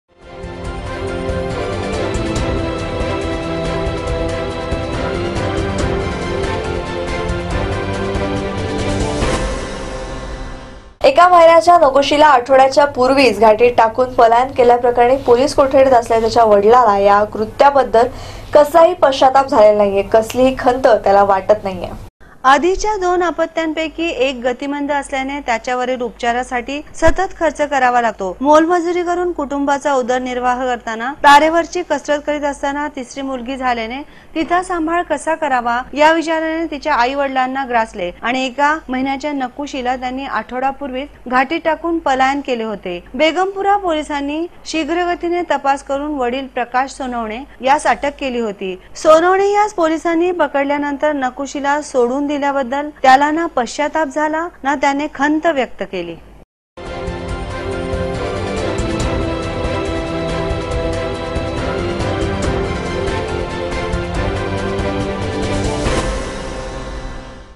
एक महिन्याच्या नकोशिला आठवड्यापूर्वीचे घाटीत टावूâन पलायन केल्याप्रकरणी पोलिस कोठडीत असलेल्या तिच्या
एक महिन्याच्या नकोशिला आठवड्यापूर्वीचे घाटीत टावूâन पलायन केल्याप्रकरणी पोलिस कोठडीत असलेल्या तिच्या वडिलाला या कृत्याबद्दल कसलाही पश्चाताप झाला नाही, कसलीही खंत वाटत नाहीये.